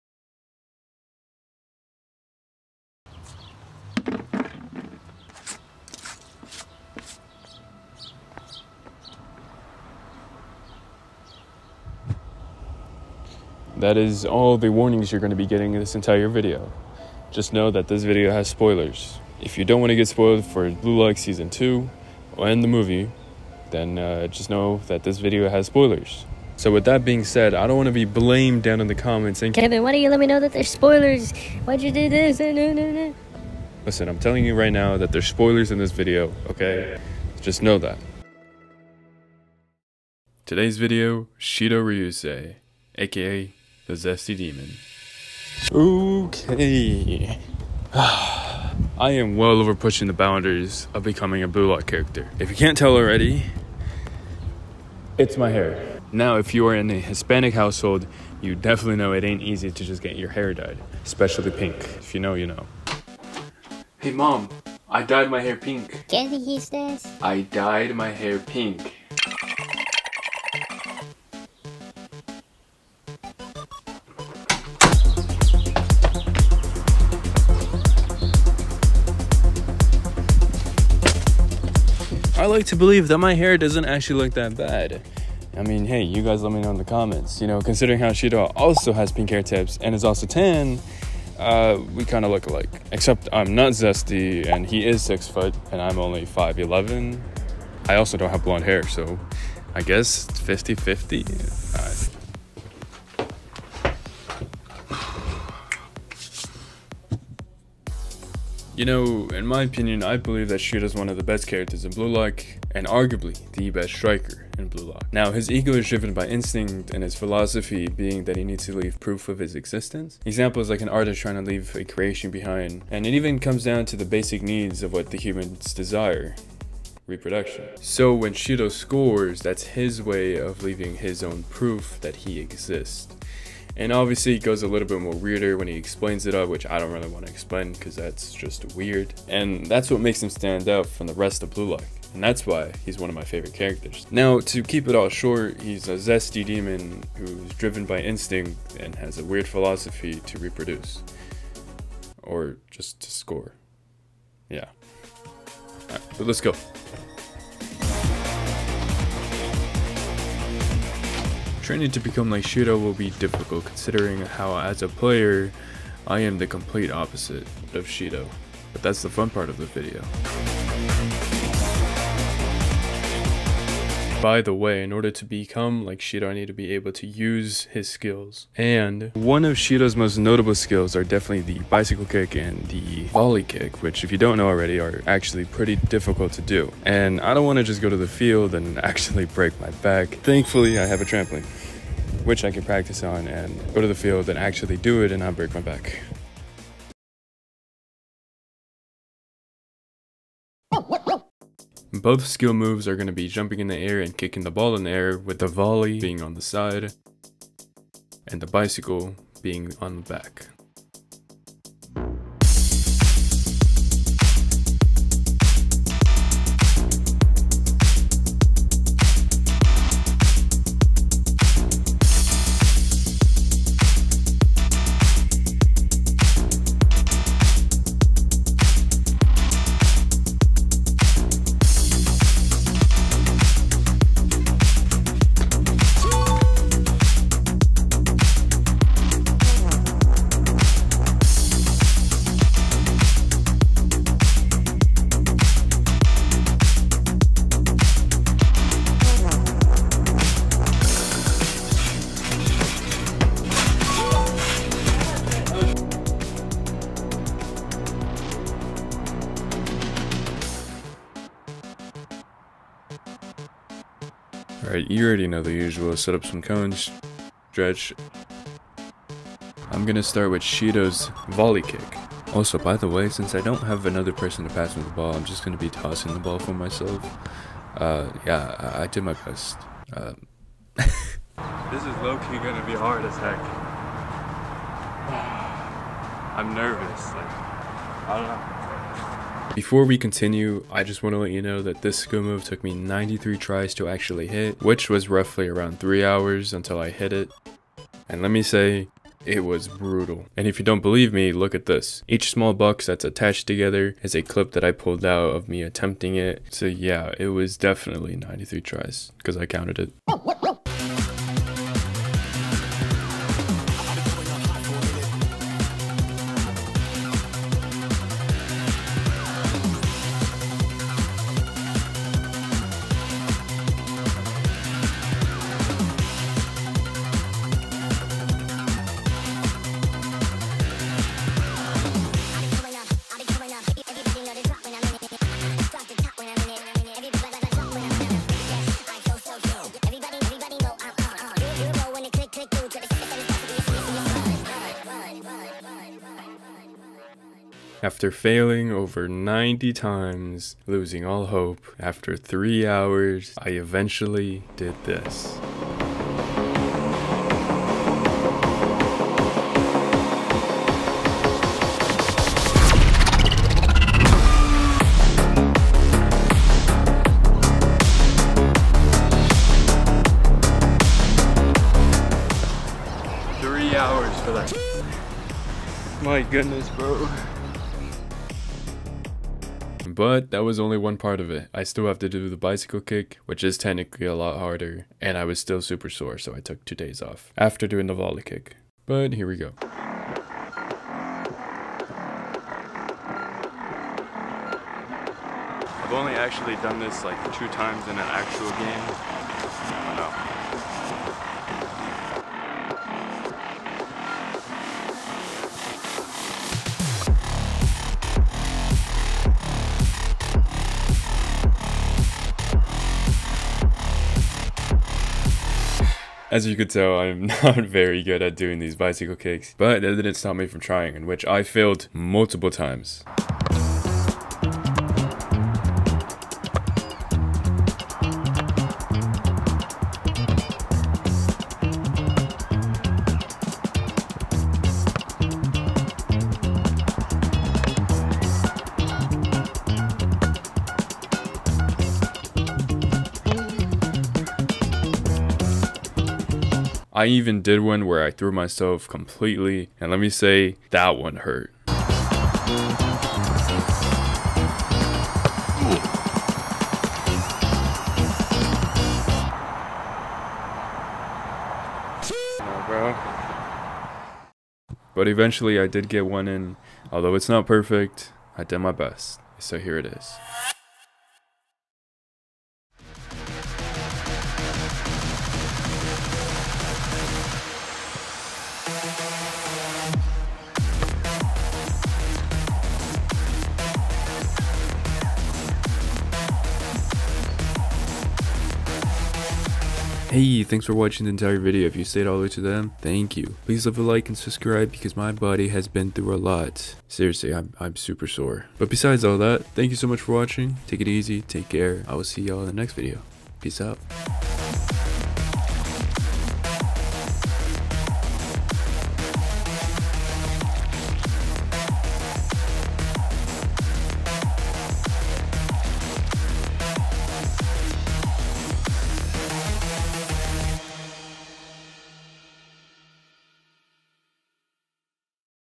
I trained to play like Shidou Ryusei | BLUE LOCK
I really tried becoming like Shidou. *Hint* I dyed my hair lol... I'm obviously not as zesty as Shidou, but that doesn't mean I can't try his skill moves. My entire body as gone through a lot for the video, so hopefully you enjoyed the vid :)